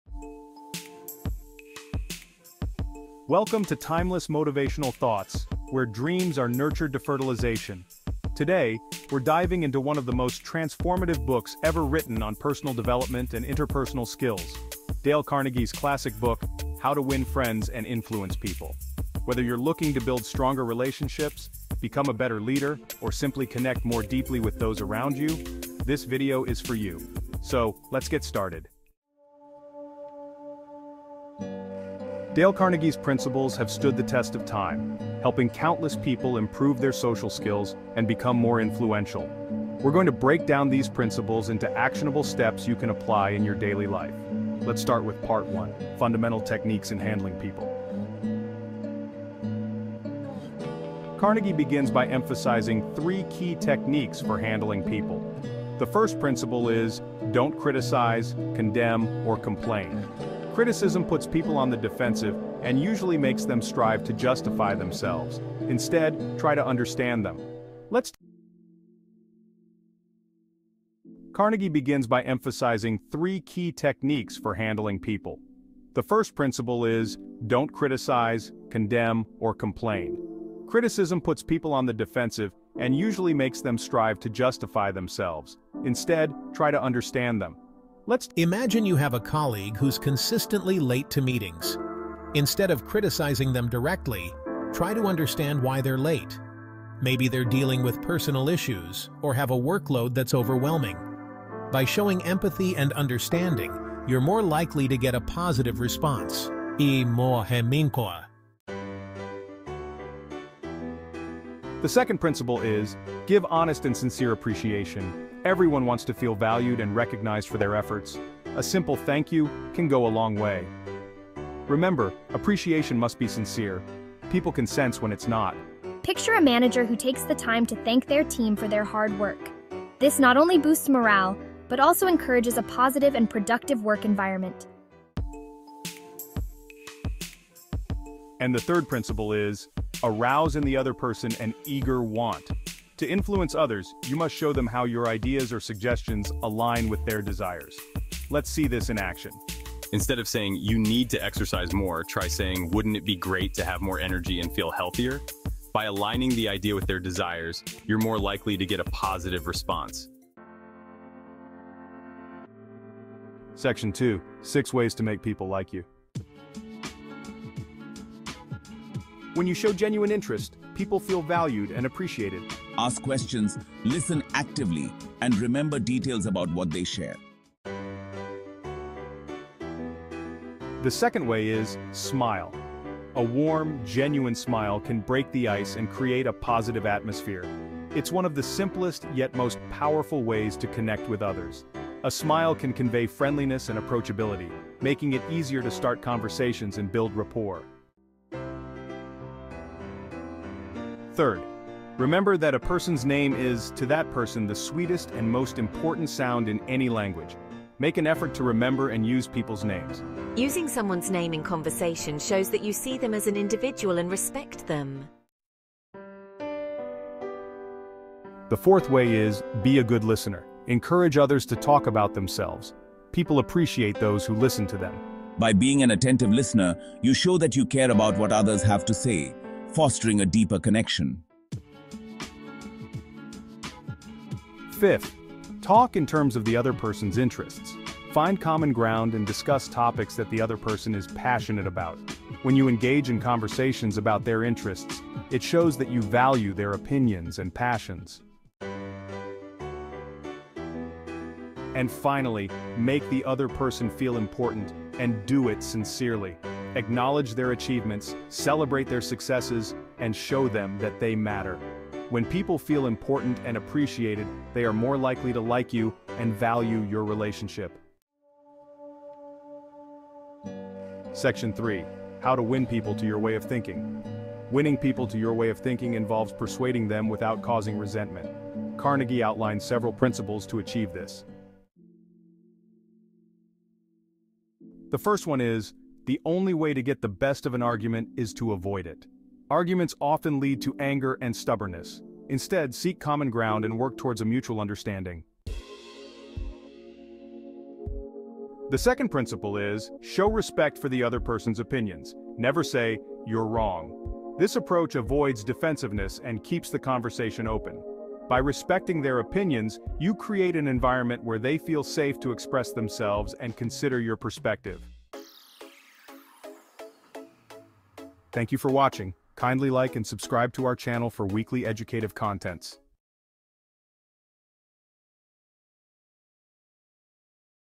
How To Win Friends and Influence People (Powerful Motivational video)
Unlock the secrets to becoming more influential and creating lasting relationships with the timeless principles from Dale Carnegie's classic, "How to Win Friends and Influence People." Discover the importance of personal development and enhancing your interpersonal skills. Learn actionable steps to apply Carnegie's principles like showing genuine interest, avoiding criticism, and appreciating others sincerely. See how aligning ideas with others' desires can lead to positive responses and how respect and active listening can build strong connections. Boost your social skills and transform your life today!

OUTLINE:

00:00:00 Introduction
00:00:28 How to Win Friends and Influence People
00:00:47 Fundamental Techniques in Handling People
00:01:08 Don't Criticize, Condemn, or Complain
00:01:48 Give Honest and Sincere Appreciation
00:02:15 Arouse in the Other Person an Eager Want
00:02:40 Six Ways to Make People Like You
00:02:46 Show Genuine Interest
00:02:57 Smile
00:03:17 Remember Names
00:03:35 Be a Good Listener
00:03:51 Talk in Terms of the Other Person's Interests
00:04:08 Make the Other Person Feel Important
00:04:27 How to Win People to Your Way of Thinking
00:04:41 Avoid Arguments
00:04:55 Show Respect for the Other Person's Opinions
00:05:15 Conclusion